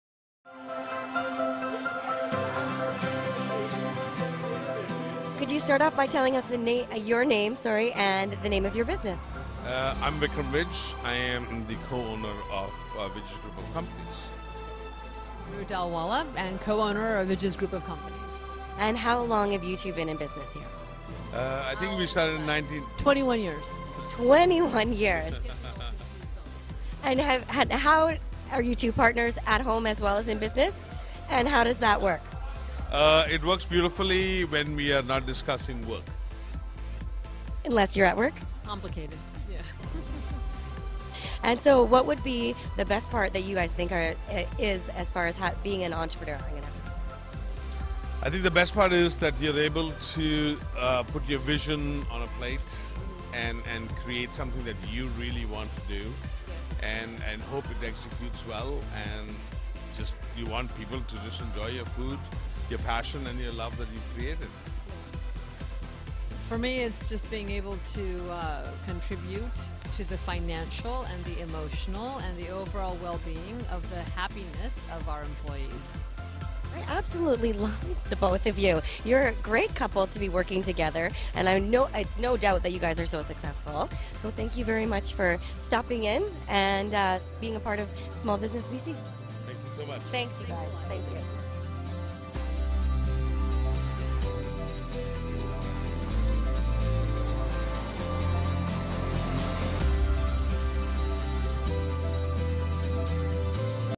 SBBC Awards - Vij’s Restaurant on LifeStyle Channel
About Vij’s Restaurant

We opened Vij’s restaurant in Vancouver, BC in autumn 1994 with the premise that there is no such thing as a typical Indian curry. Each of our meals showcases the beauty and taste of different Indian spices in combination with locally available ingredients. Our innovative and unique style of cuisine has won numerous awards from 1995 to present.